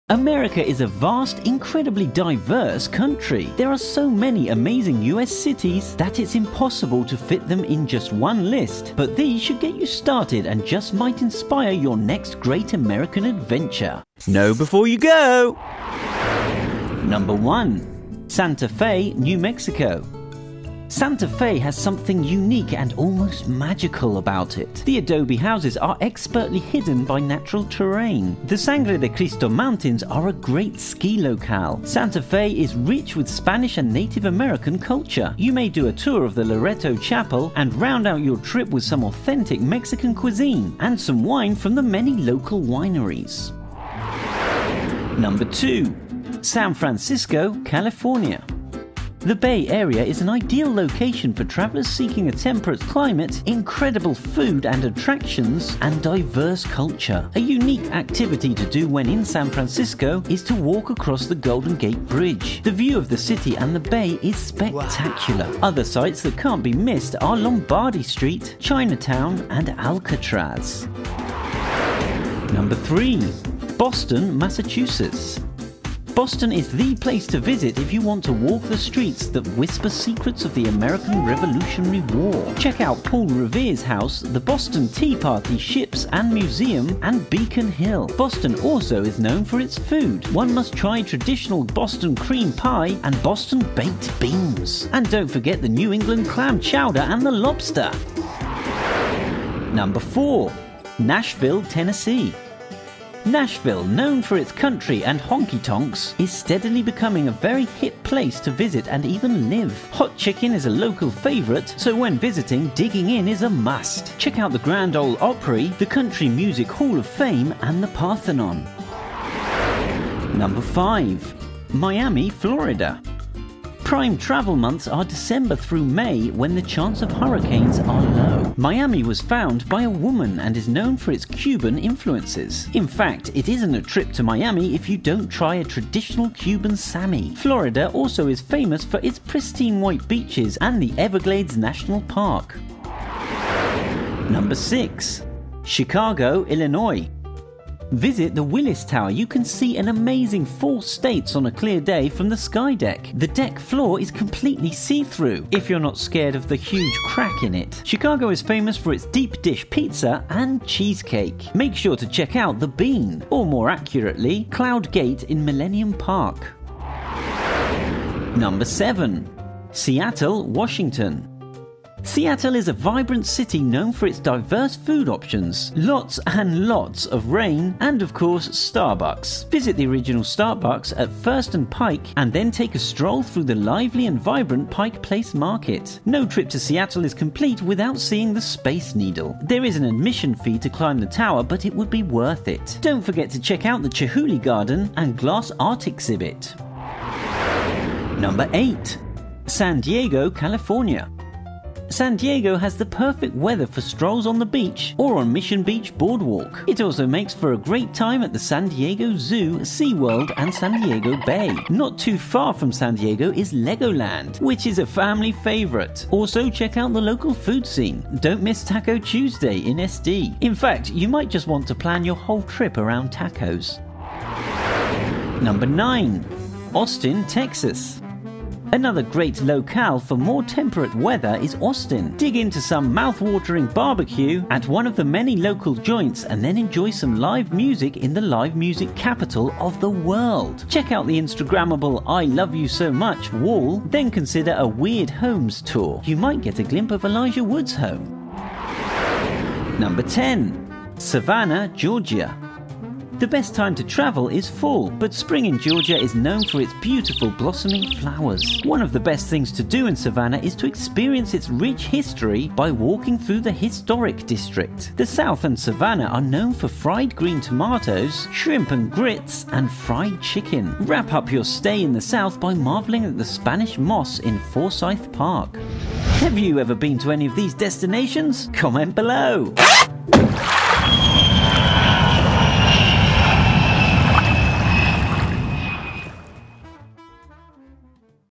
10 Best Cities to Visit in the US in 2019
America is a vast, incredibly diverse country. There are so many amazing US cities, that it's impossible to fit them in just one list but these should get you started and just might inspire your next great American adventure.

1 SANTA FE, NEW MEXICO
Santa Fe has something unique and almost magical about it. The adobe houses are expertly hidden by natural terrain, the Sangre de Cristo Mountains are a great ski locale. Santa Fe is rich with Spanish and Native American culture. You may do a tour of the Loretto Chapel and round out your trip with some authentic Mexican cuisine and some wine from the many local wineries.

2 SAN FRANCISCO, CALIFORNIA
The Bay Area is an ideal location for travelers seeking a temperate climate, incredible food and attractions, and diverse culture. A unique activity to do when in San Francisco is to walk across the Golden Gate Bridge, the view of the city and the bay is spectacular.
Other sights that can’t be missed are Lombardi Street, China Town and Alcatraz.

3 BOSTON, MASSACHUSETTS
Boston is the place to visit if you want to walk the streets that whisper secrets of the American Revolutionary War. Check out Paul Revere’s house, the Boston Tea Party Ships and Museum, and Beacon Hill. Boston also is known for its food, one must try traditional Boston Cream Pie and Boston Baked Beans and don’t forget the New England clam chowder and the lobster!

4 NASHVILLE, TENNSESSEE
Nashville, known for its country and honky-tonks, is steadily becoming a very hip place to visit and even live. Hot chicken is a local favorite, so when visiting, digging in is a must. Check out the Grand Ole Opry, the Country Music Hall of Fame, and the Parthenon.

5 MIAMI, FLORIDA
Prime travel months are december through may when the chance of hurricanes are low. Miami was found bya a woman and is known for its Cuban influences. In fact, it isn’t a trip to Miami if you don’t try a traditional Cuban sammie. Florida also is famous for its pristine white beaches and The Everglades National Park.

6 CHICAGO, ILLINOIS
Visit the Willis Tower, you can see an amazing four states on a clear day from the Skydeck, the deck floor is completely see-through! Chicago is famous for its deep dish pizza and cheesecake. Make sure to check out the “bean,” or more accurately, Cloud Gate in Millennium Park.

7 SEATTLE, WASHINGTON
Seattle is a vibrant city known for diverse food options, lots and lots of rain, and, of course, Starbucks. Visit the original Starbucks at 1st and Pike and then take a stroll through the lively and vibrant Pike Place Market. No trip to Seattle is complete without seeing the Space Needle. There is an admission fee to climb the tower but it be worth it. Do not forget check out the Chihuly Garden and Glass art exhibit.

8 SAN DIEGO, CALIFORNIA
San Diego has the perfect weather for strolls on the beach or on Mission Beach Boardwalk. also makes for a great time at the San Diego Zoo, SeaWorld, and San Diego Bay. Not too far from San Diego is Legoland, which is a family favorite.
Check out the local food scene. Do not miss Taco Tuesday in SD. In fact, you might just want to plan your trip around tacos.

9 AUSTIN, TEXAS
Another great locale for more temperate weather is Austin. Dig into some mouth-watering barbeque at one of the many local joints and then enjoy some live music in the “Live Music Capital of the World.” Check out the Instagrammable I Love You So Much wall then consider a “weird homes” tour. You might get a glimpse of Elijah Wood’s home.

10 SAVANNAH, GEORGIA
The best time to travel is fall, but spring in Georgia is known for its beautiful blossoming flowers. One of the best things to do in Savannah is to experience its rich history by walking through the Historic District. The south and Savannah are known for fried green tomatoes, shrimp and grits, and fried chicken. Wrap up your stay in the south by marveling at the Spanish moss in Forsyth Park.

Have you ever been to any of these destinations?